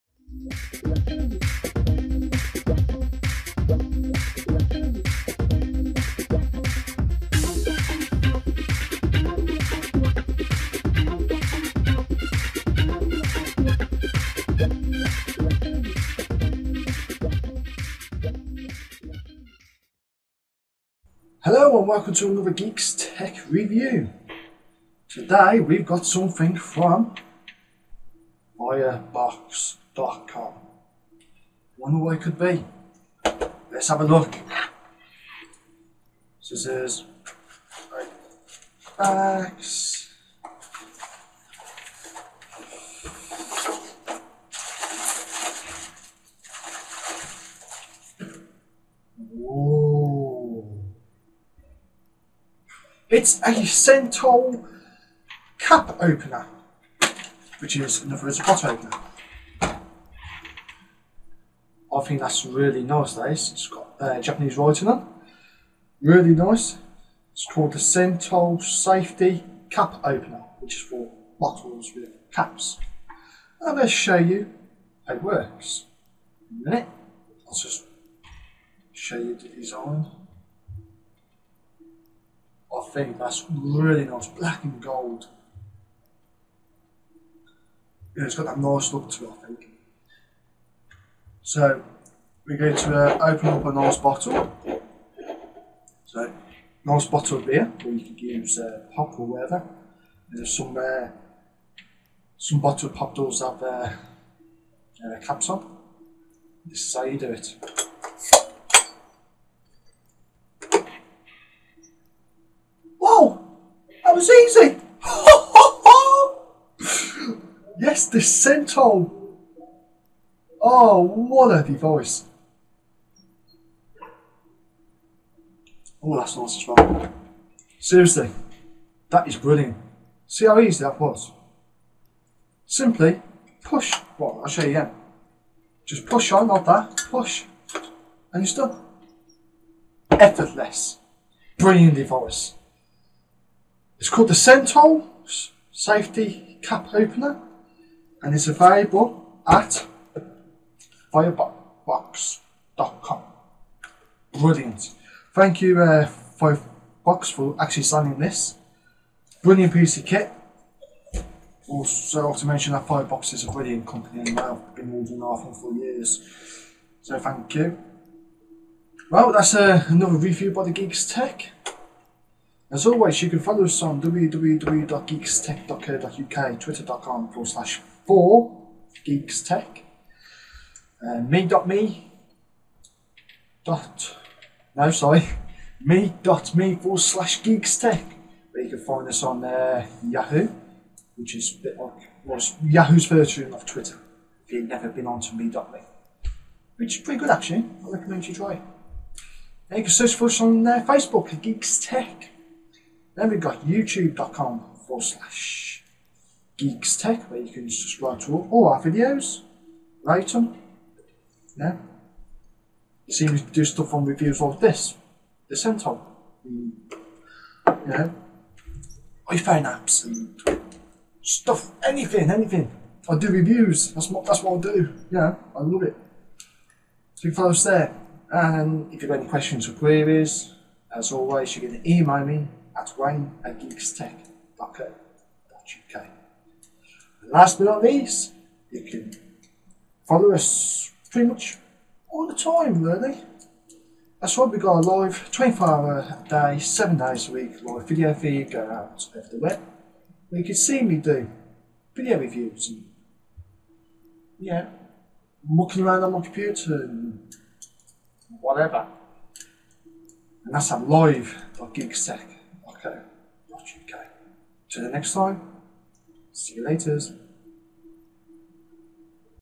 Review: Sentol bottle opener
This is what gadgets are all about. Sure, you can open your beer with any old bottle opener but you certainly won't generate a crowd of onlookers all desperate for a go.

Sentol bottle openers are the original push down bottle openers.

Incredibly simple to use, even when the operator has had one too many